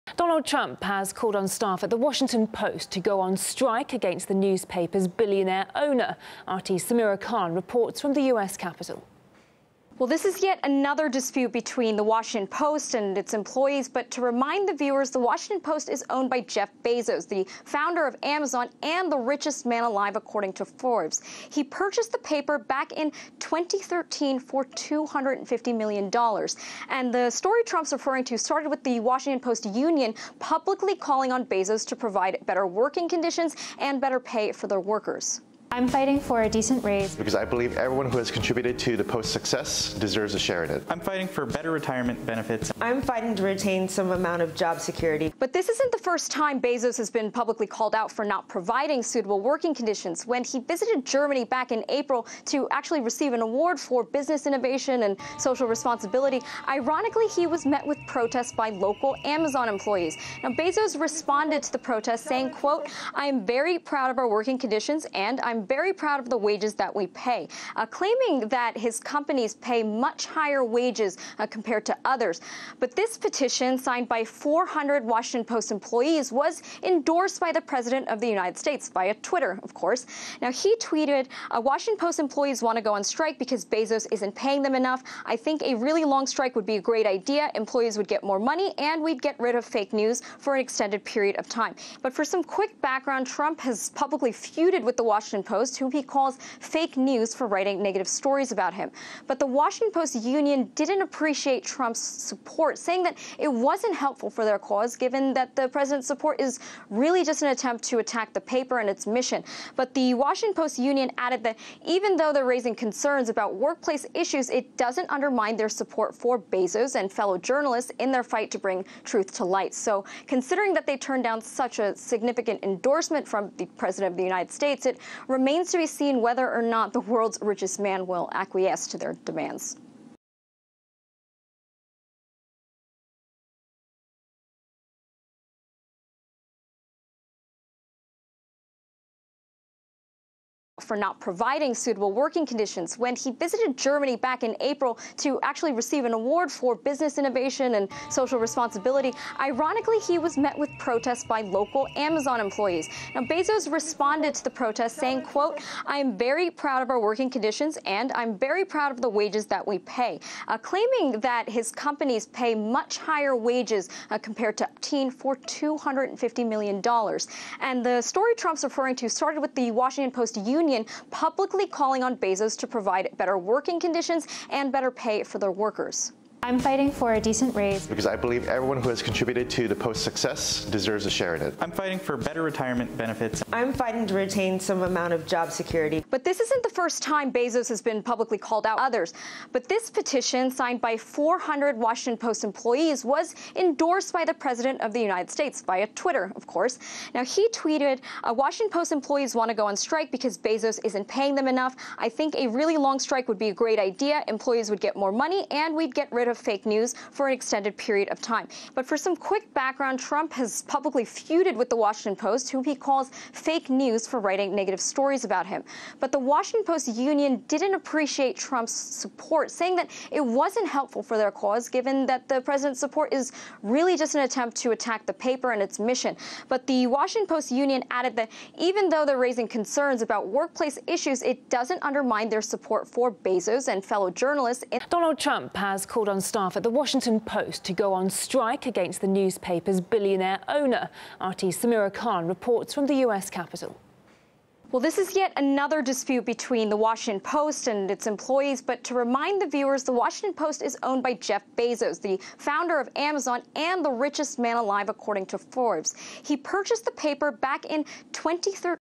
Workers of WaPo, Unite! Trump calls for newspaper staff to strike
Donald Trump has called on staff at the Washington Post to go on strike against the newspaper's billionaire owner. Check out RT LIVE